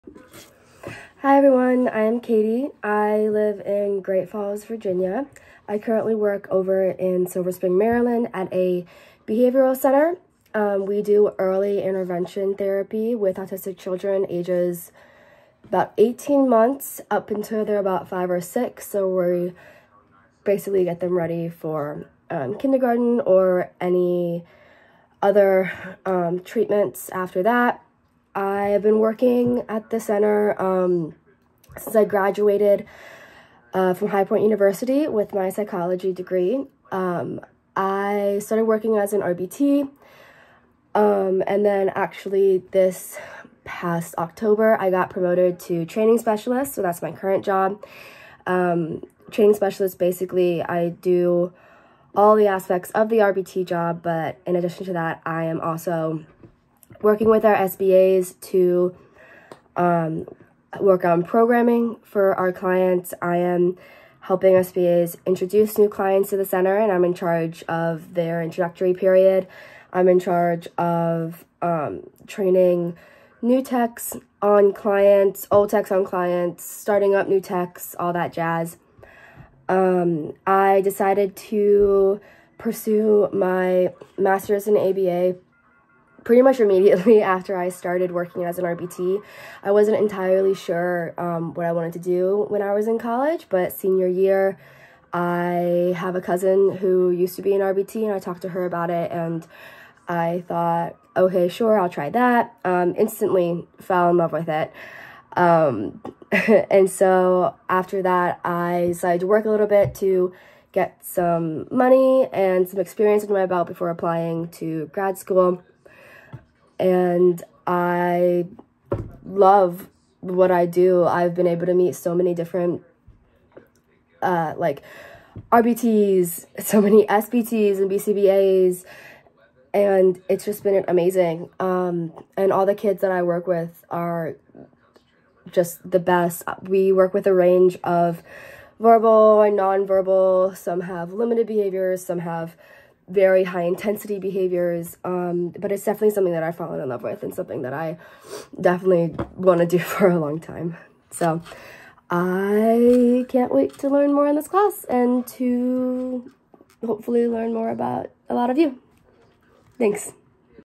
Introduction Video
Hi all! I am so excited to be apart of this class and can't wait to learn more about you all!